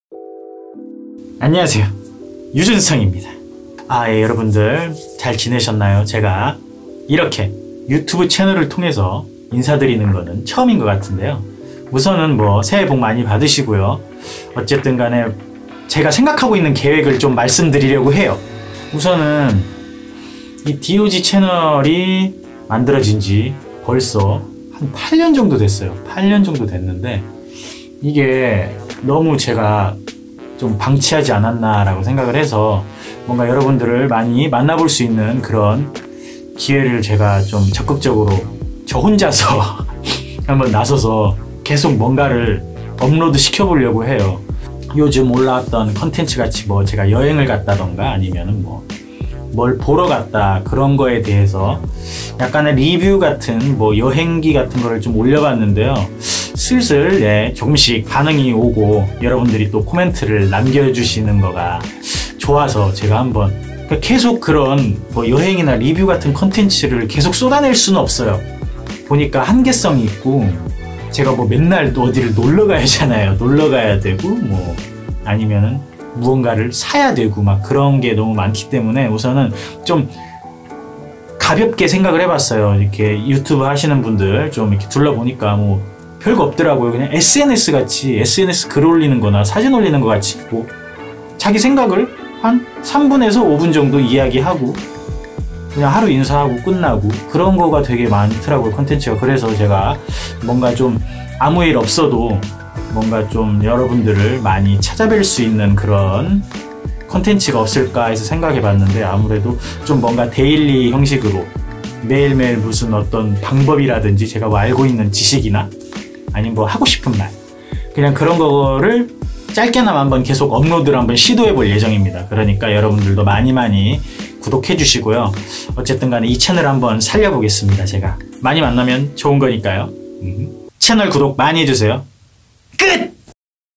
유준성의 채널 인사 및 앞으로 계획
죽거가는 채널의 심장에 다시 불을 지펴 볼 예정입니다
많이 사랑해 주세용~~ㅋ

유준성 솔로 앨범 링크